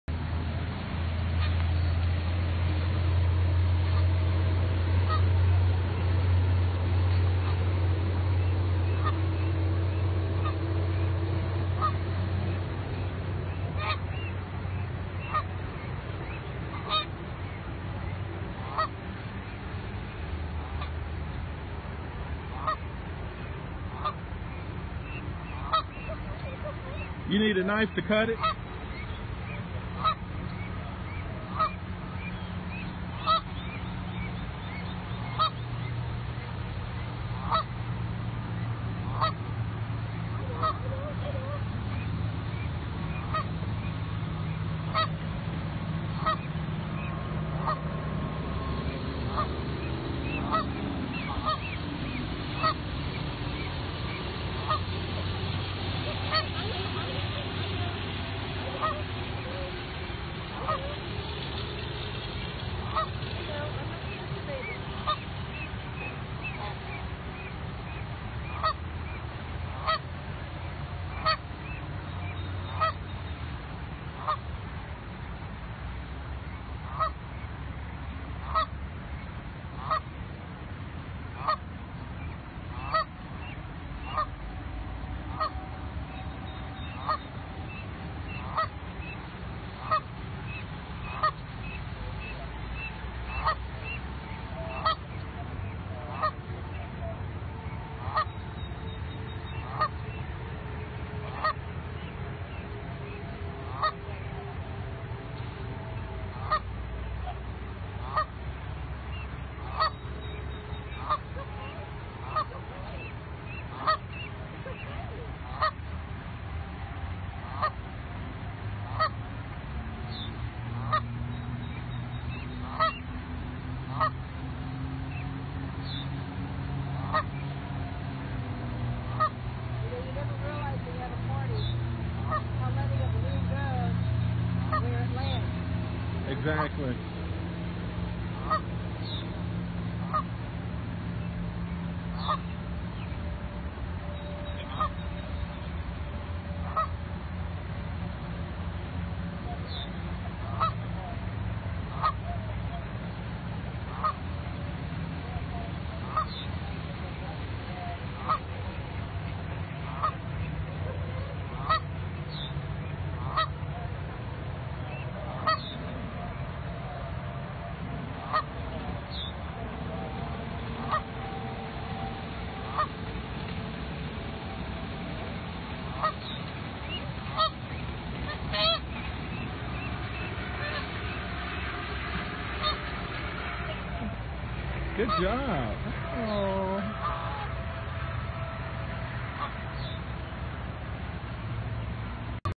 Cincinnati police officers save baby goose from balloon
The city's finest helped out a fine feathered friend Monday when officers untangled a gosling who was caught near Mill Creek on what appears to be a balloon string. Video via James Givens.